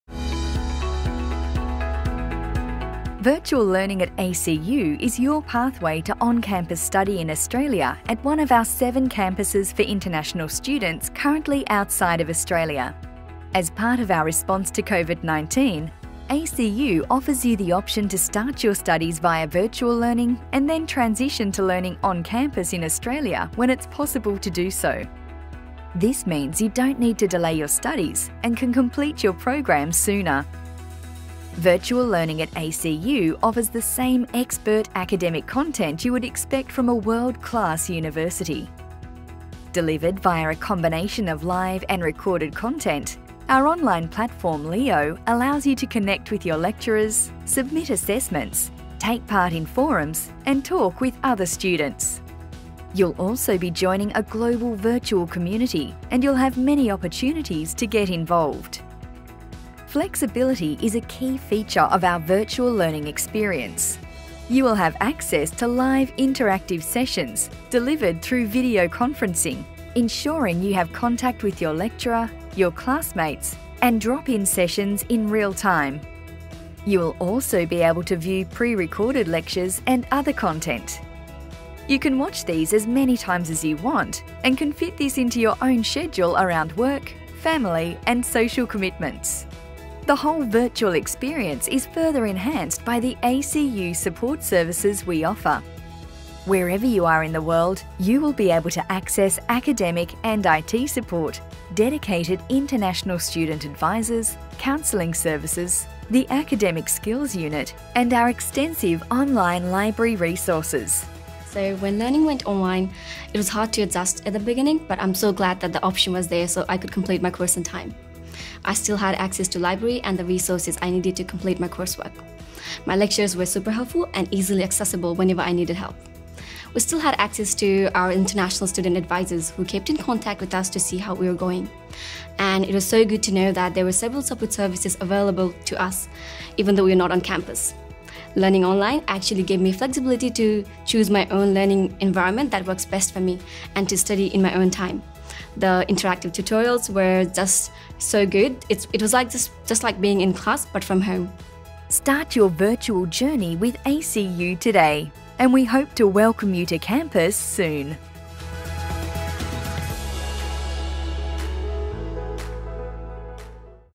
Virtual Learning at ACU for International Students
Learn about what to expect from virtual learning as an ACU international student studying in your home country.